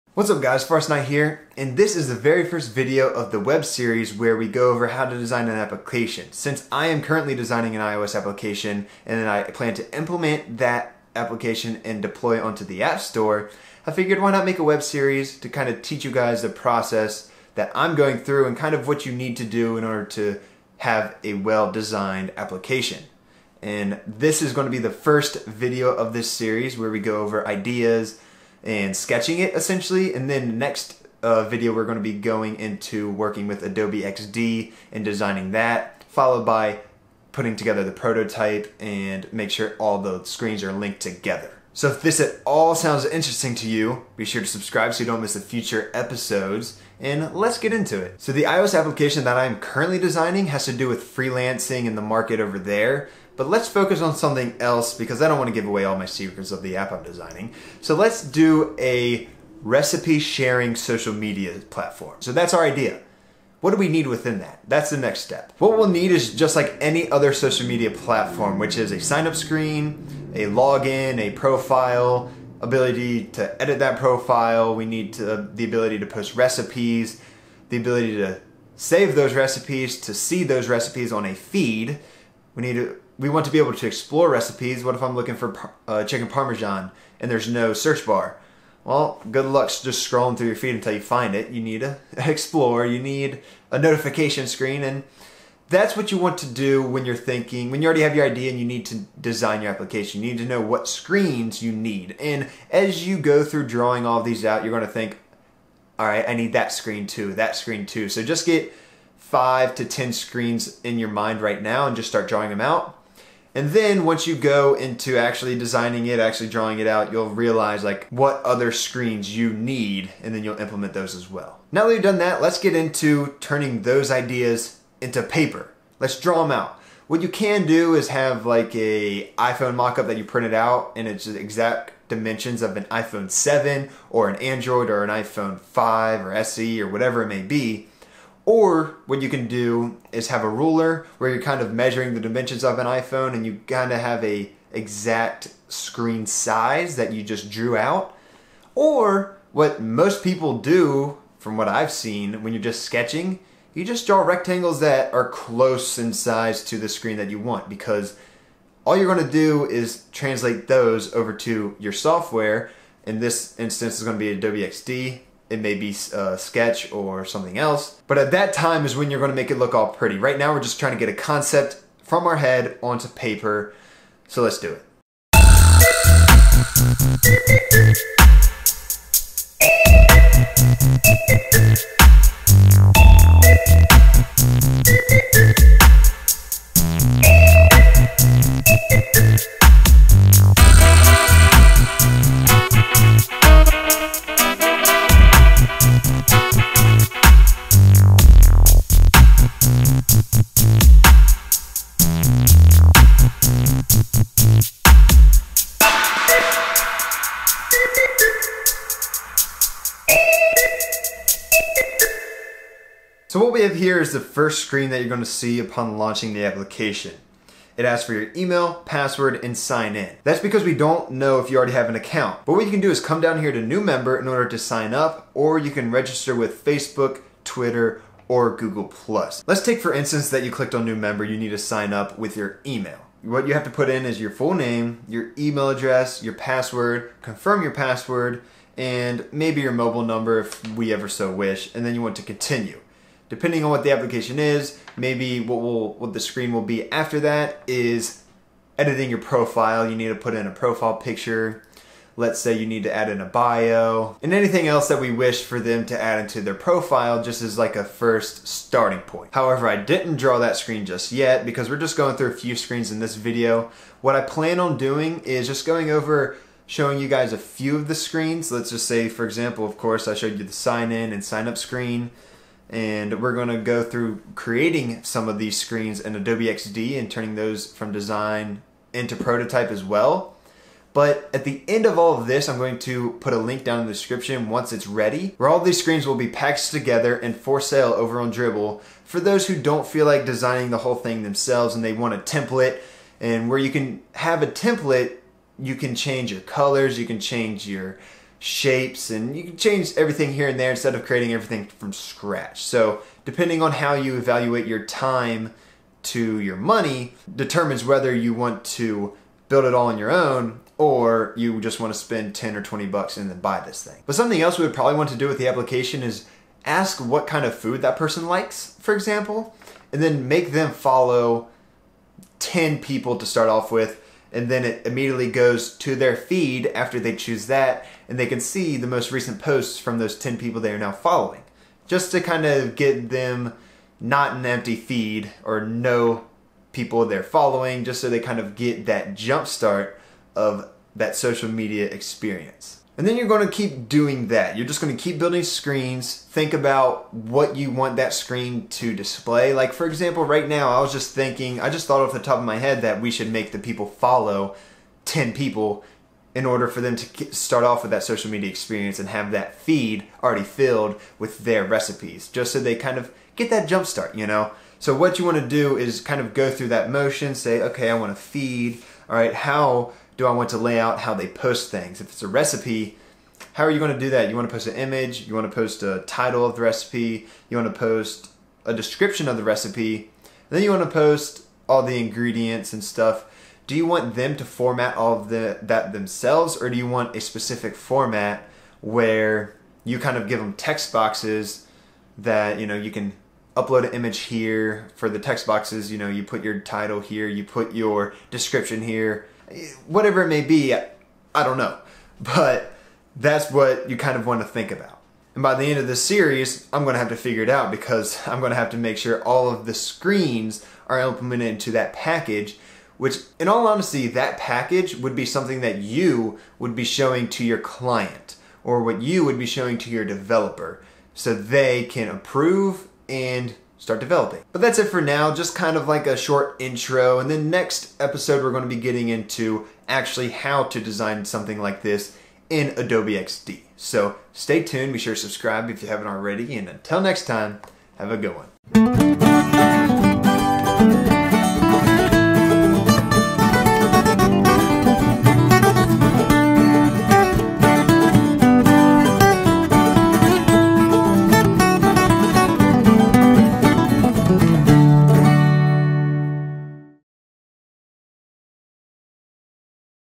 How to Design an App (iOS or Android) - Ideas and Sketches
Design is the very first step into turning your mobile app idea into reality. However, one must have an idea in order to create a mobile application. That's what we discuss in this video.

Camera - Panasonic Lumix G7

Microphone - Rode Video Micro

Tripod - Manfrotto Compact Action Tripod

Bendy Tripod - Joby GorillaPod

LINKS

Udemy iOS 10 & Swift 3 Course (JUST ENROLLED, I DIDN'T MAKE IT)